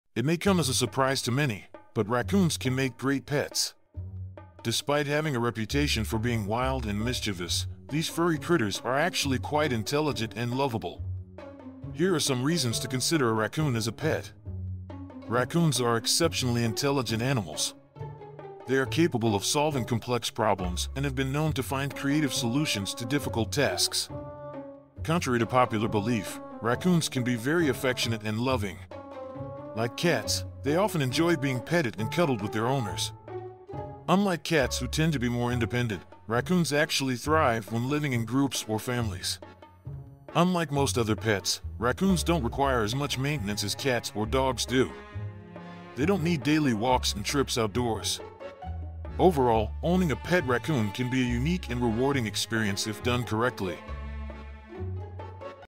Raccoon 🦝 One Alternative Animal To Have As A Pet
Raccoon 🦝 One Alternative Animal To Have As A Pet Raccoons make great pets?! Despite their notorious reputation for being mischievous, these furry critters are actually quite intelligent and lovable. In this video, we discuss five reasons why raccoons could be the perfect pet for you. From their intelligence and affection to their social nature and low-maintenance lifestyle - raccoons can make a unique and rewarding companion if given the proper care and attention. So, if you're looking for an unconventional pet that will give you unconditional love, why not consider a raccoon?